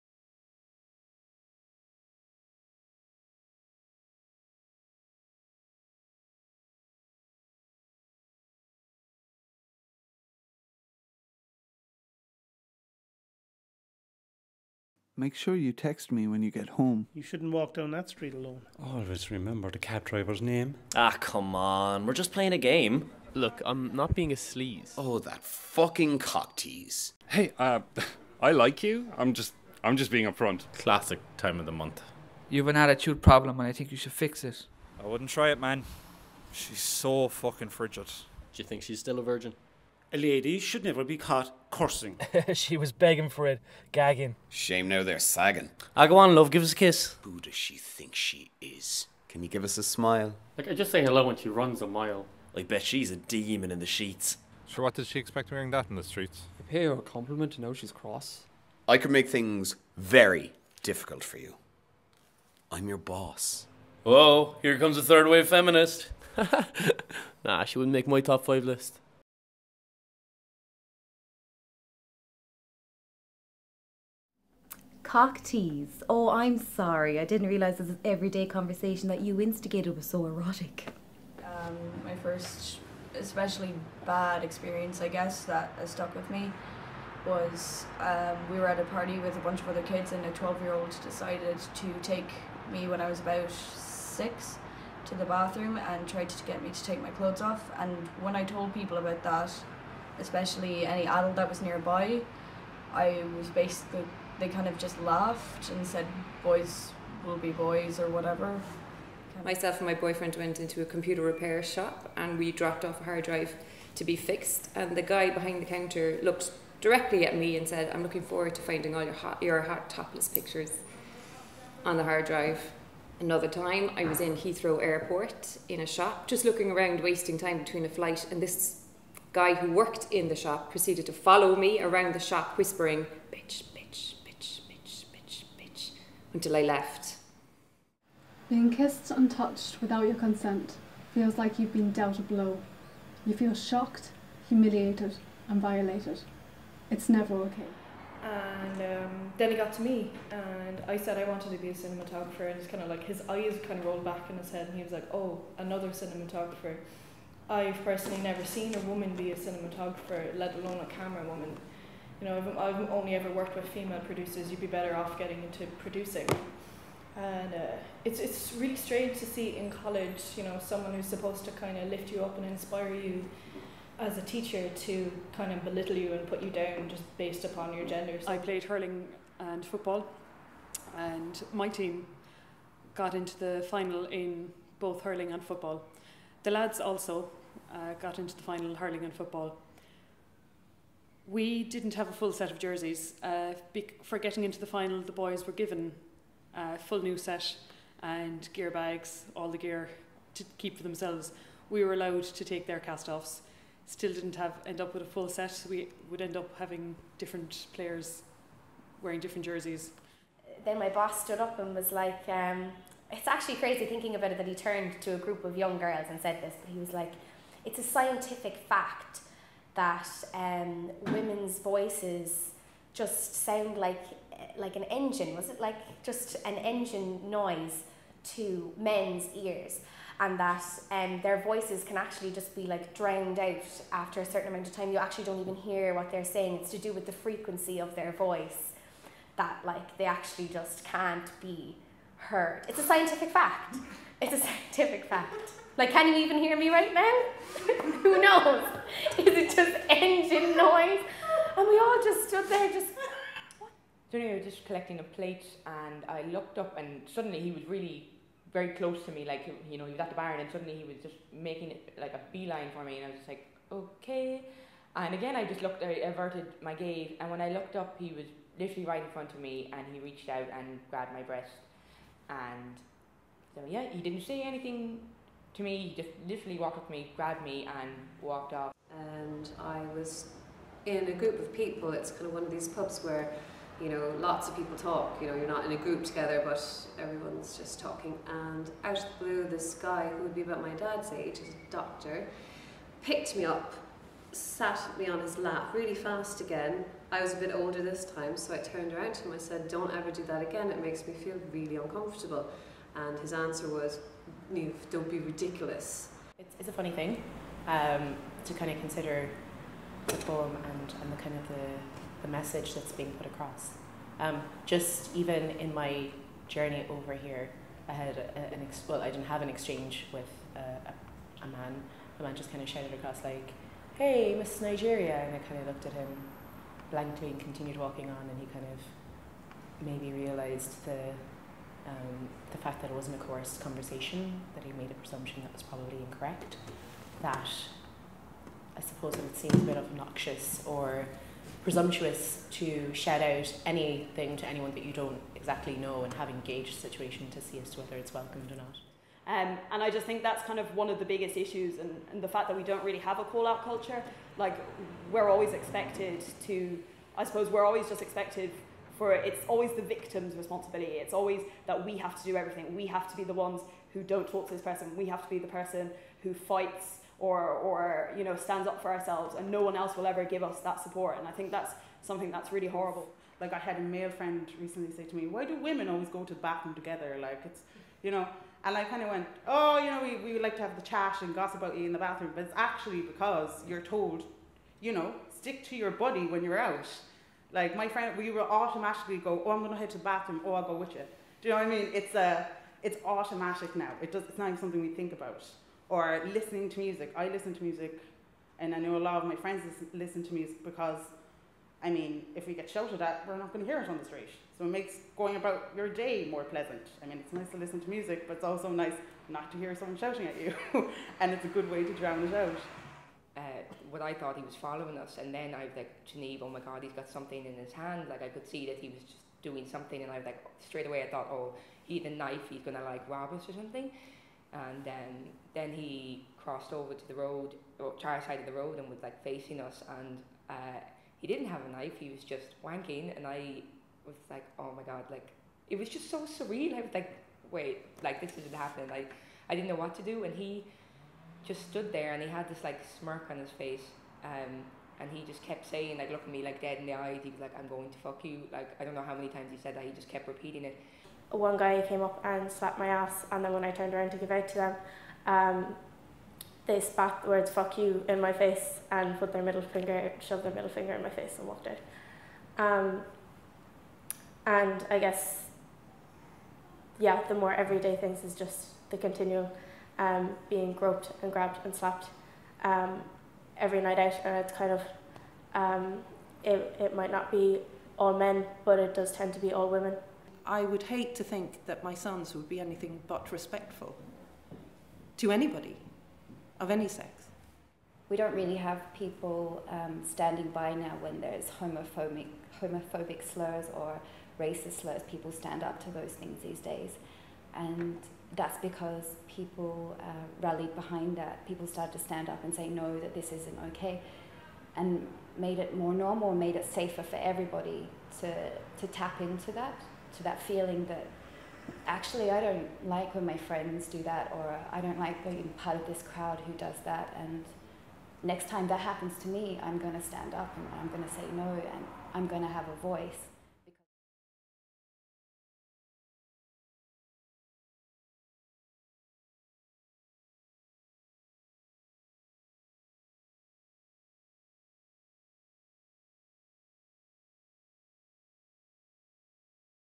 Someone You Know
A poem tackling sexism and women sharing their harrowing stories of how they face it in their everyday life.

Directed by Deep Red
Poem written by Emmet O'Brien

Thanks to all the women who shared their stories:

Niamh Kavanagh
Jodie Burns
Claire Loy
Claire Lamb
Katy Dunne-Magner
Tara O'Donnovan
Chloe O'Reilly
Maeve Quaid
Niamh Murphy
Senita Appiakorang
Kelly Doherty
Mary Keating
Deirdre Quaid
Judy Donovan
Mary Pappin

And our thanks to the actors who said horrible things

Jerry Coughlan
David Nelligan
Stephen Broekhuizen
Phillip M. Connolly
Emmet O'Brien
Seán Mac Gearailt
Stephen O'Connor
Aaron O'Neill
Brendan Morris
Darragh Keating
Cúán Breathnach
Cethan Leahy
David O'Leary
Christopher O'Flaherty
Darren O'Mahony
Mike O'Dowd
Michael Browne
Niall Duggan

Special thanks to Niamh Collins, Jessica Healy and Paula Larkin.